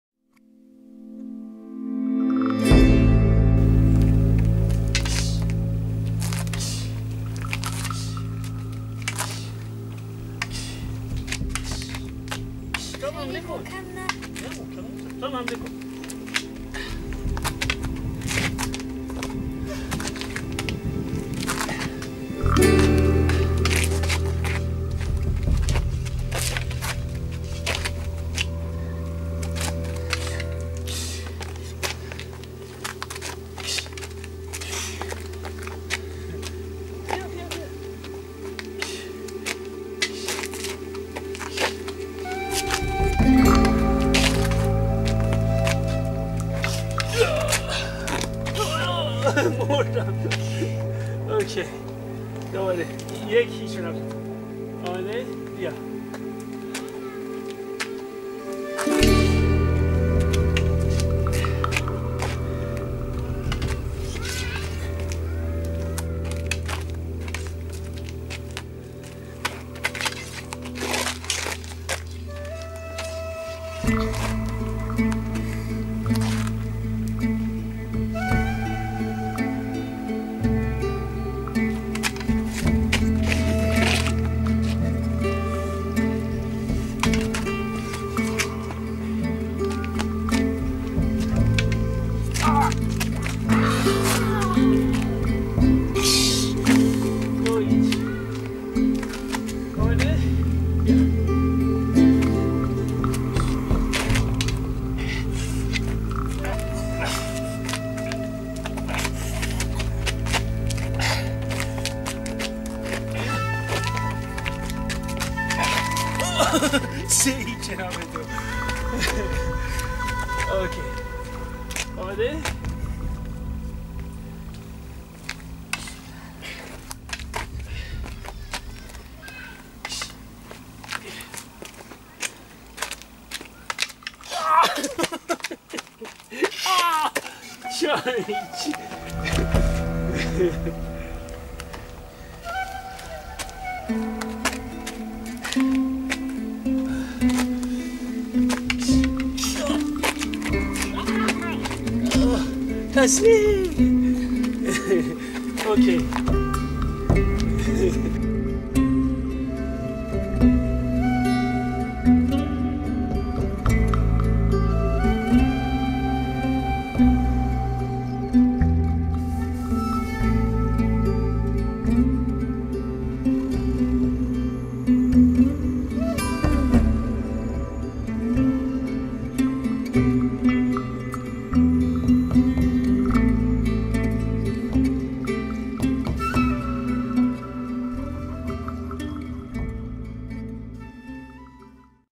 Nima (my son) and I are practicing swordsmanship with wooden swords
In this video, you will see Nima using a wooden jian and I using a wooden dao.